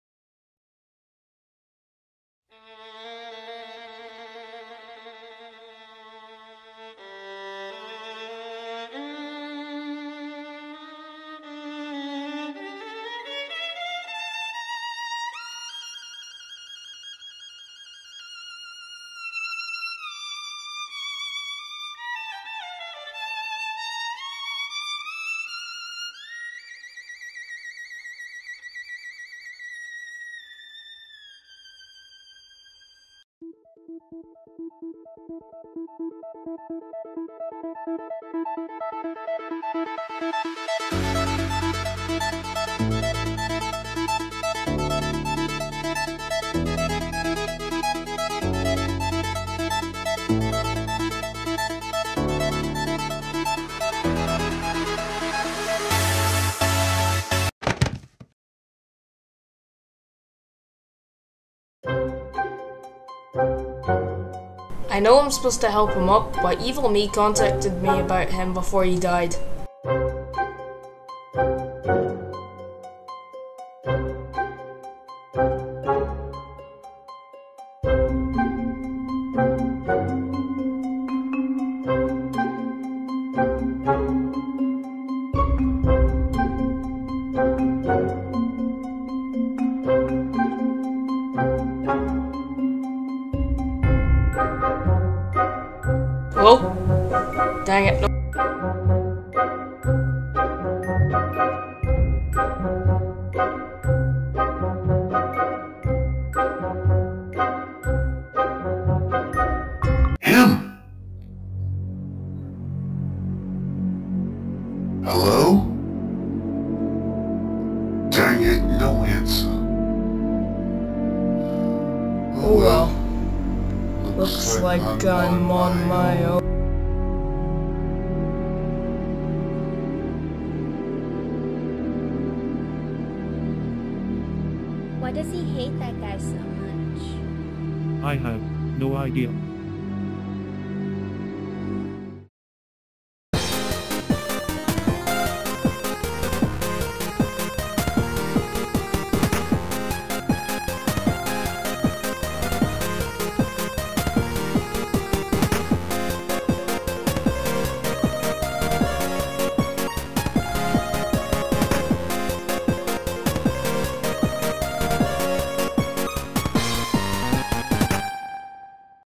The World Of Nice Episode 1 - Greetings
Finally After So Long Since Episode 7 Of The World Of Evil

Here's The World Of Nice Episode 1

Characters:
WON Evan - Me
Tri-borg - Shadow Javell98
WON Cori - SuperAlly
WON Kerr - None

Thanks For Being Patient On Me Starting This Series